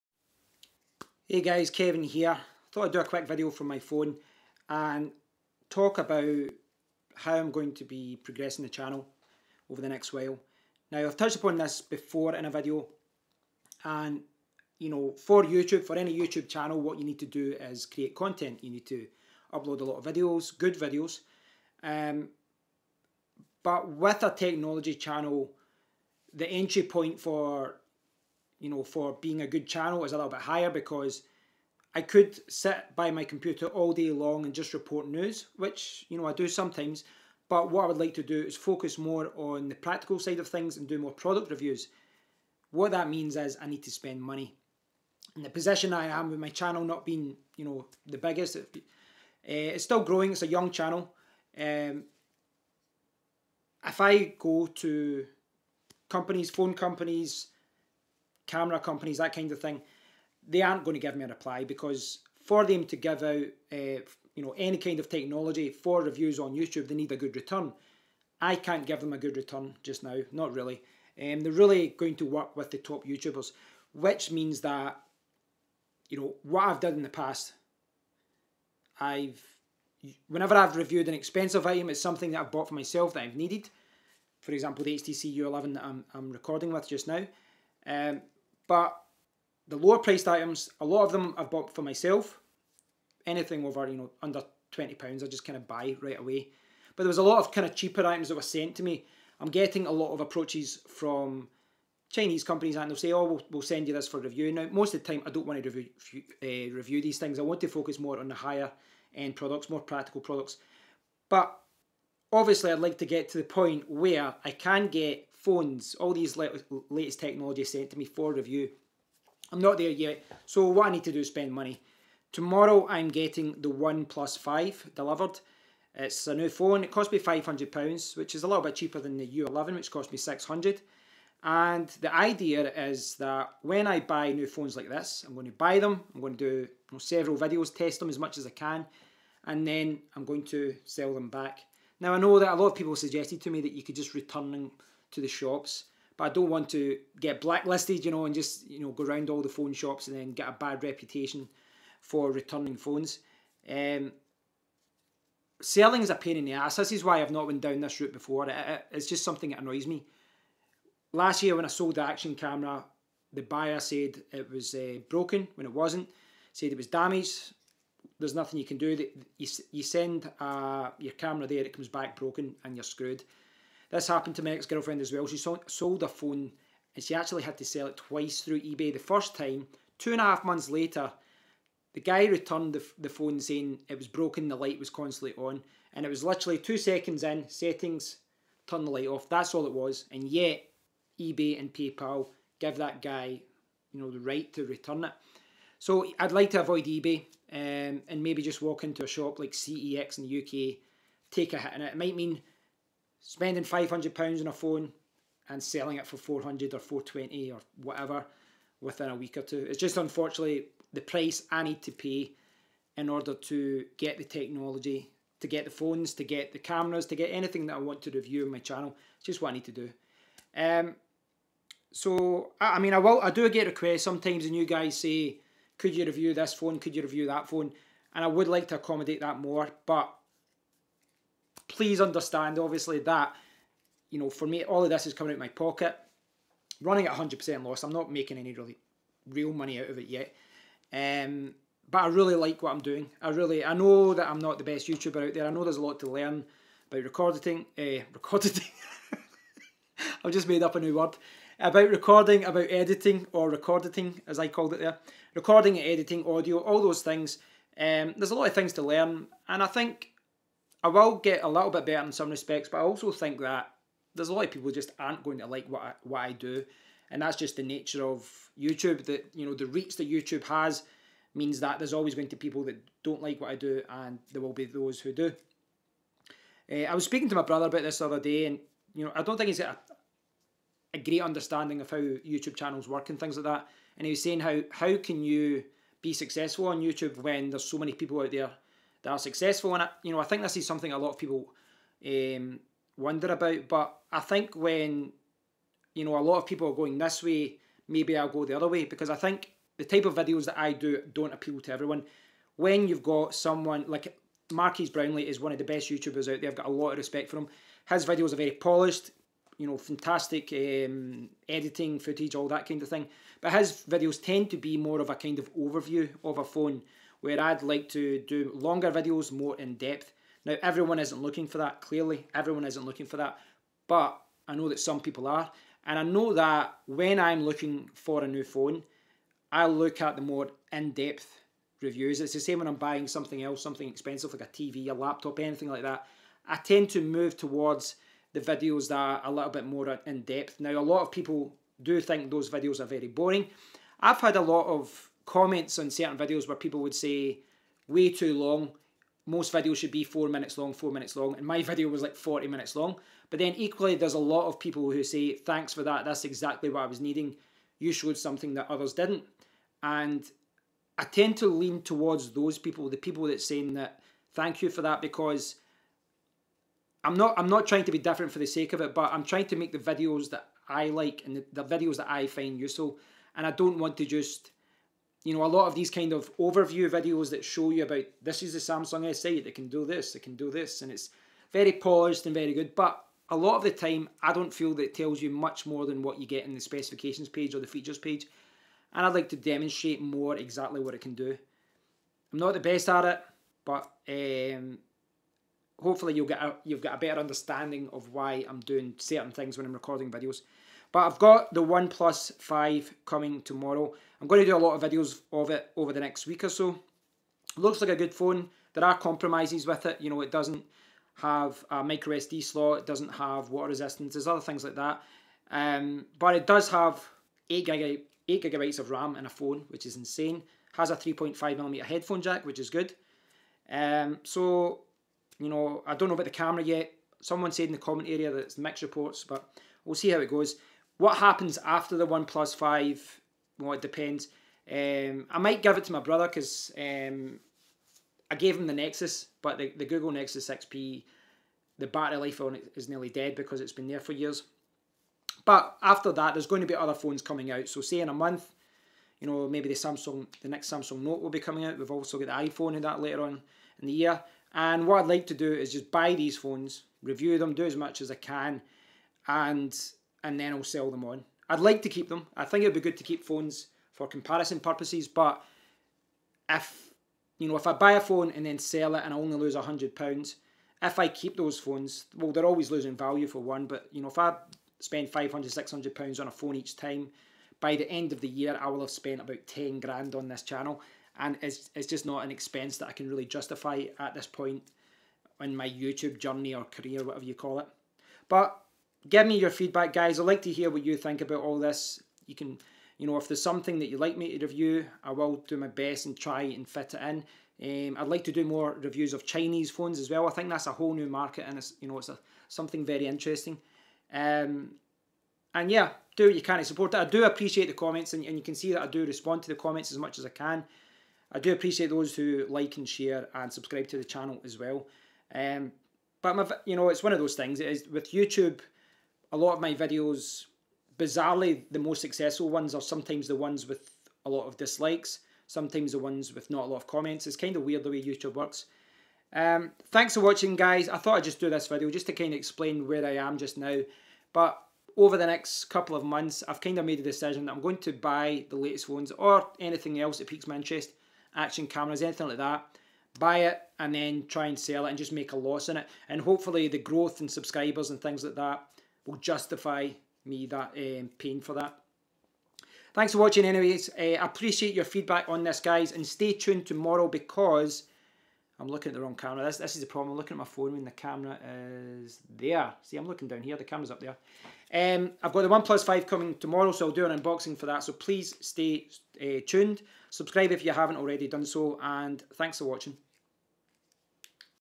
An Update on How I am Going to Develop My YouTube Channel
I thought I would explain how I am going to change the focus of my channel over the next few months.

Those who do not will be bored to tears!! :)

Thanks,
Kevin